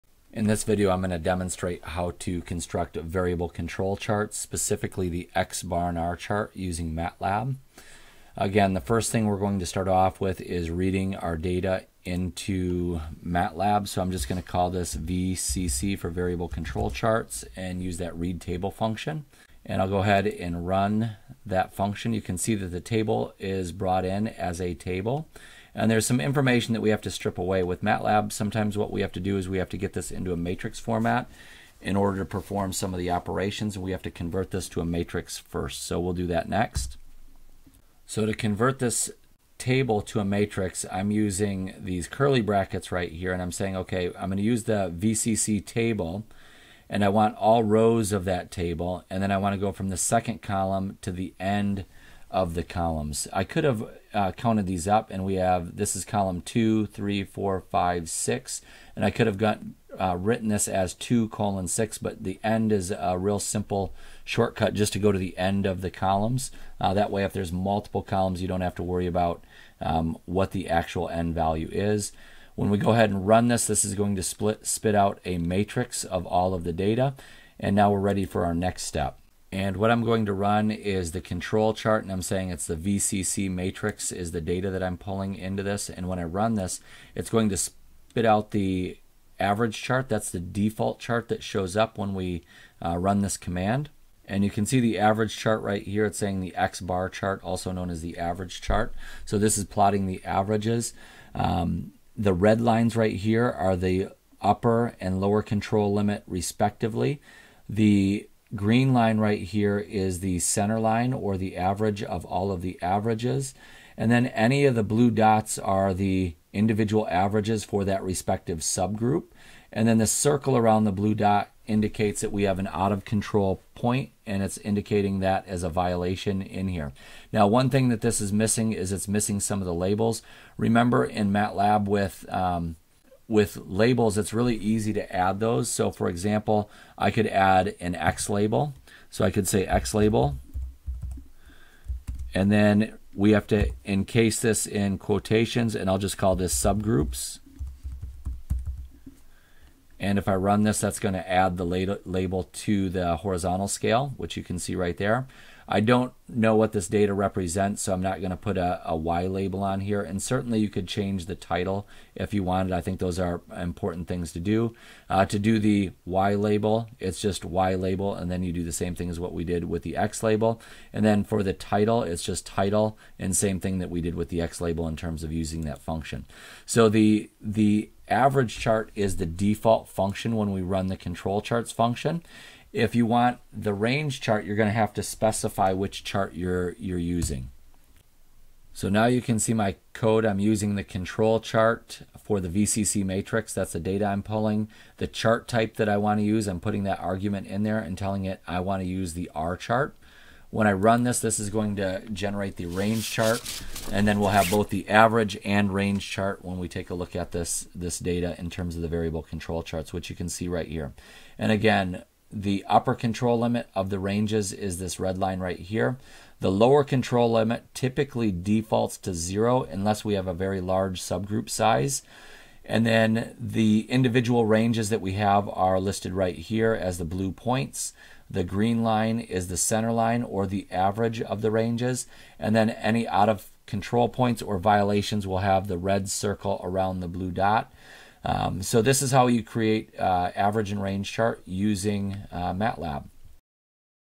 Variable Control Charts
This video shows how to construct x-bar and R charts using MATLAB.